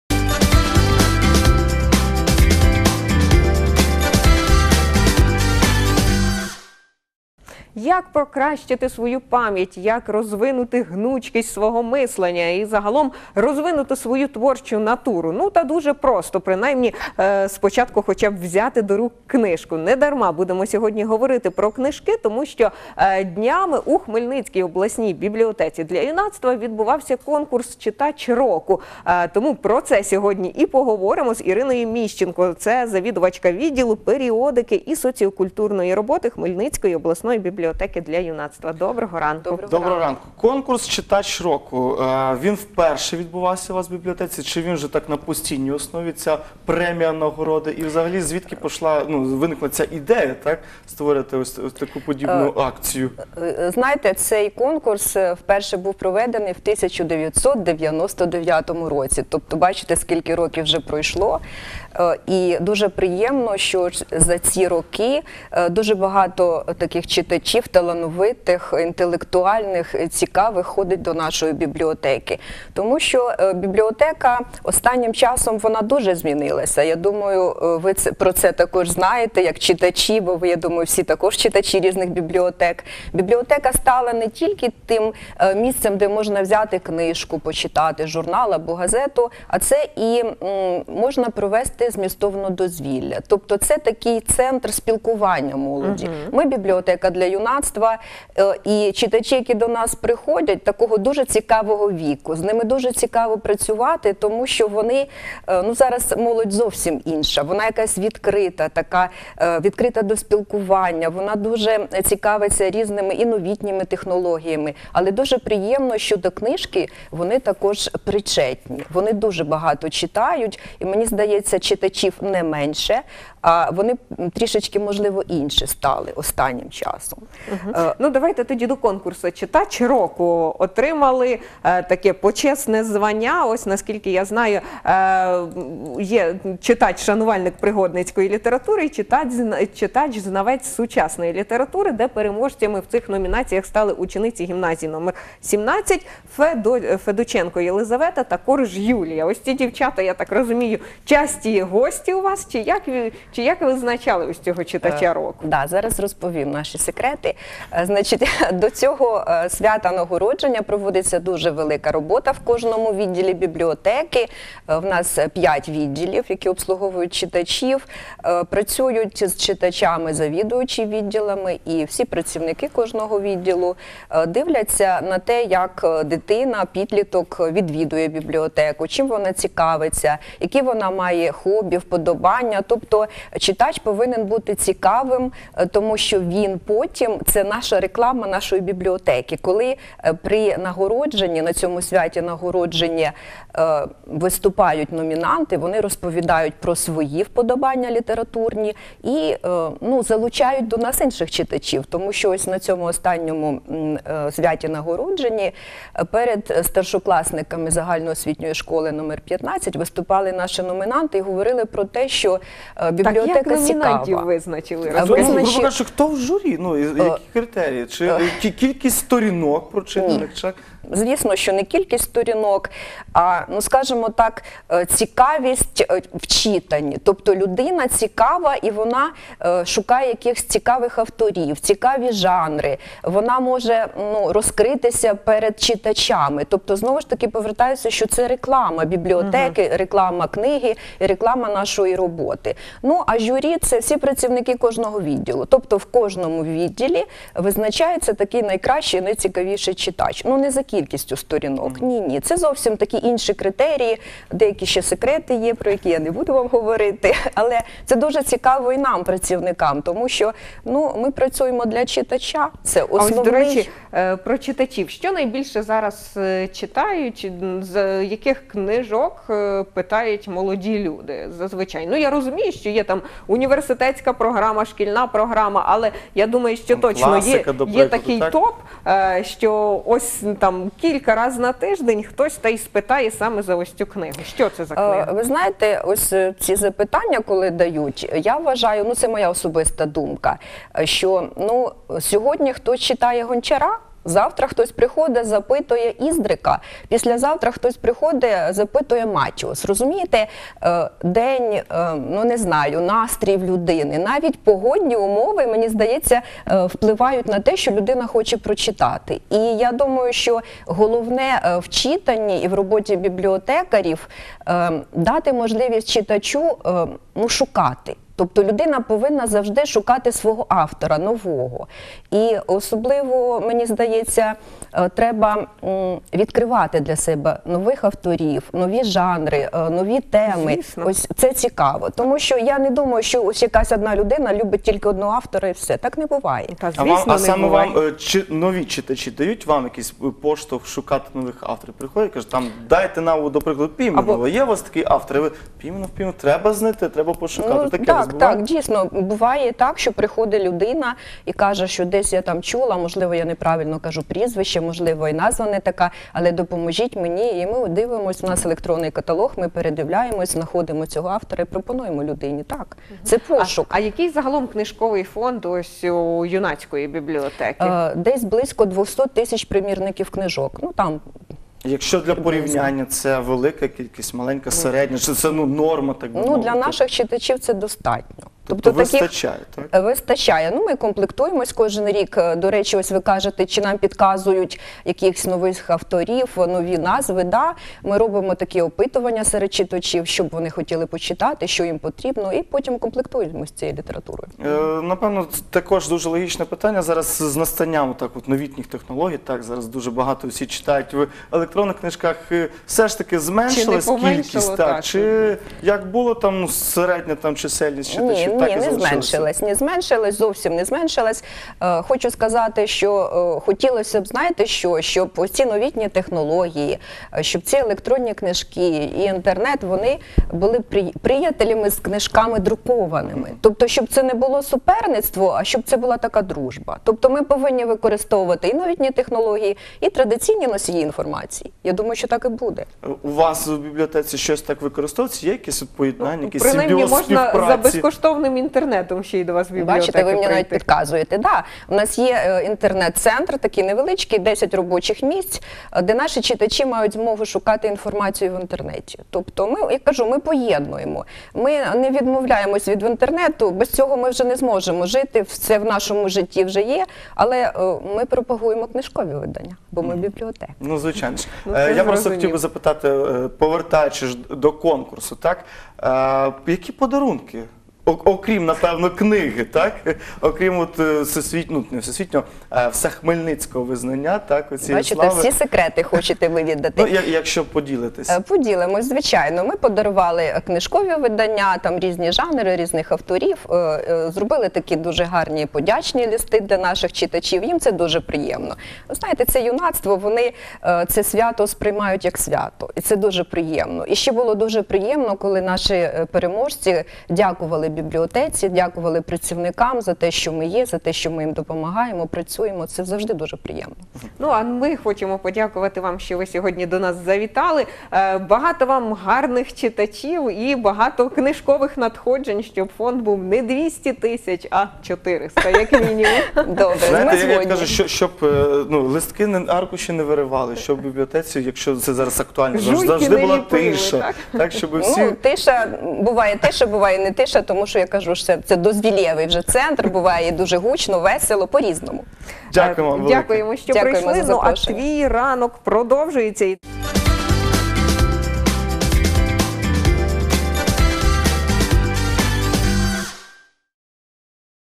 Гість на чашку кави - Ірина Міщенко
чи ходять люди до бібліотек? які книжки чатають? що нового у світі книжок? про це і не тільки, говорили з завідувачкою відділу періодики і соціокультурної роботи хмельницької обласної юнацької бібліотеки